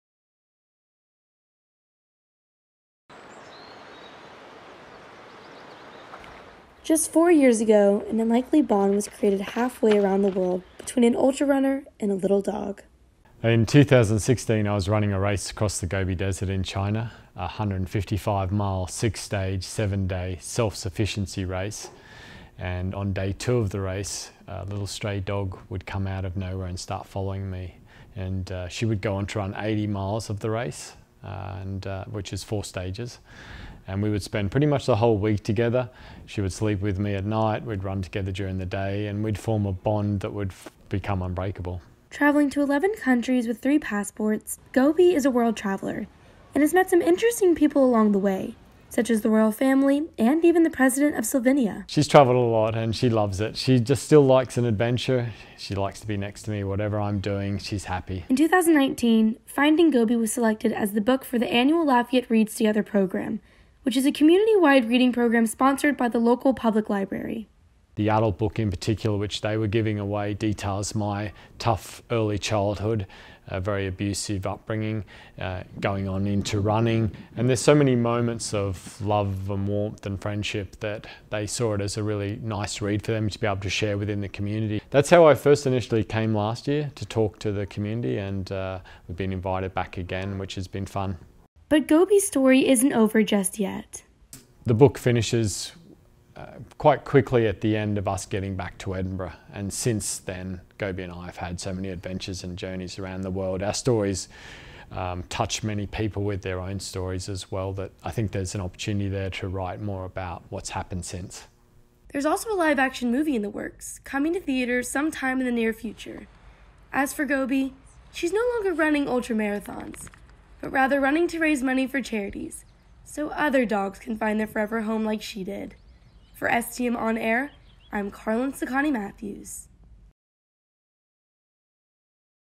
STM School media interview with Dion and Gobi
Students from St Thomas More Catholic School in Lafayette, Louisiana recently interviewed Dion and Gobi on the story of Finding Gobi.